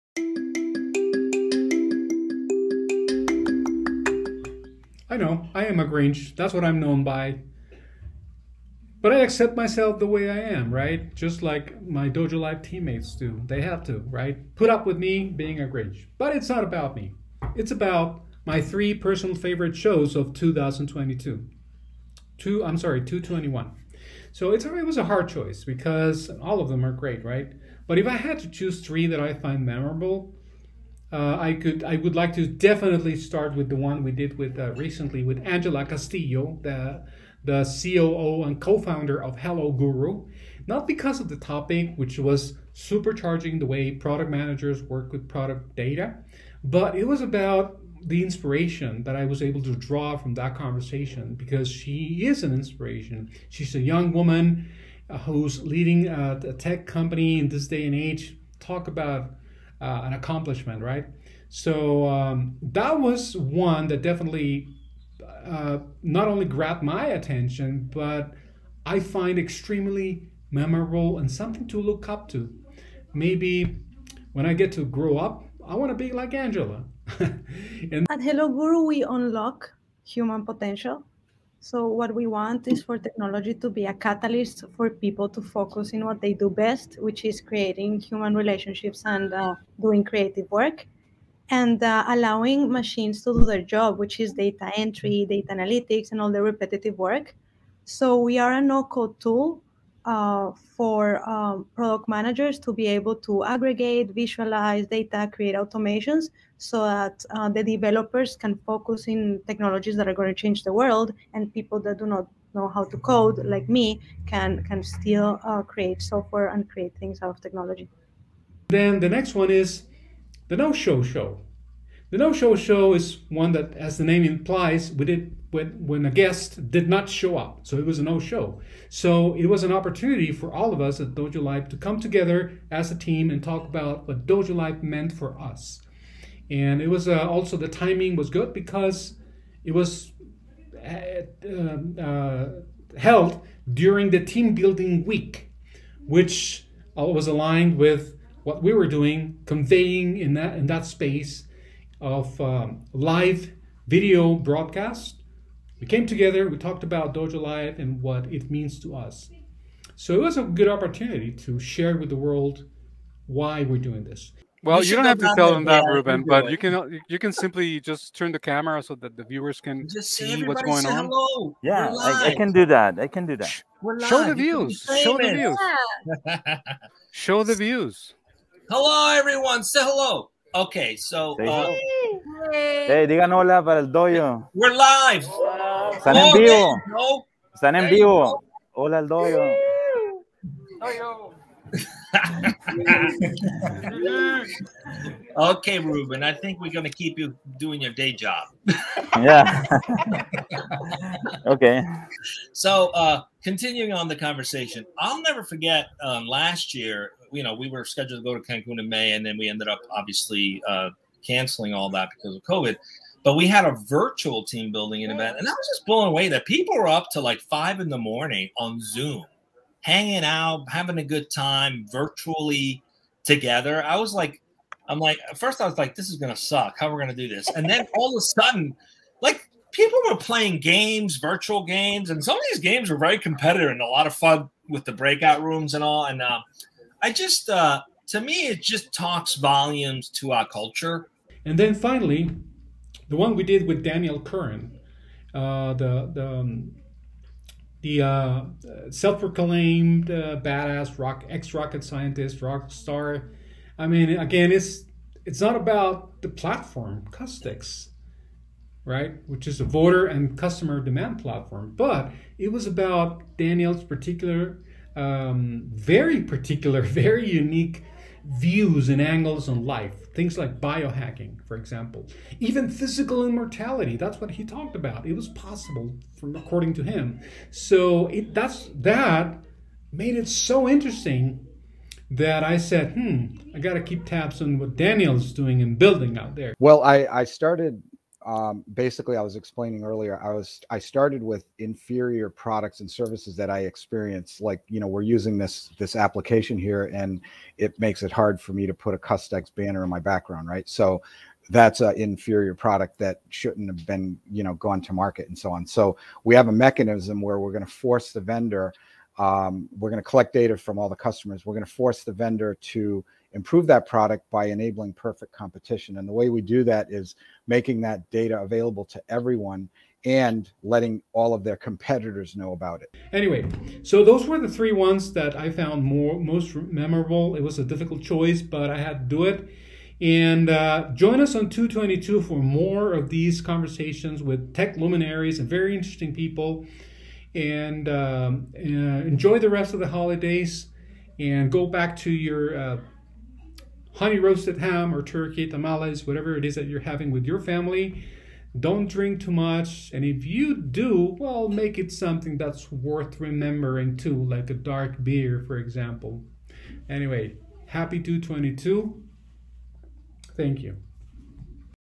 Carlos Ponce - Top 3 Shows at dojo.live
Many, many stories have been told on dojo.live thanks to our host, Carlos Ponce. 🤟

What are Carlos' top three picks of the year? Watch this video to find out!

1️⃣ "Free the Developers Using NoCode" with Angela Castillo, Co-Founder & COO @ HelloGuru

3️⃣ "Improving Products and Services for Everyone" with Daniel Curran, Founder & CEO @ CustEx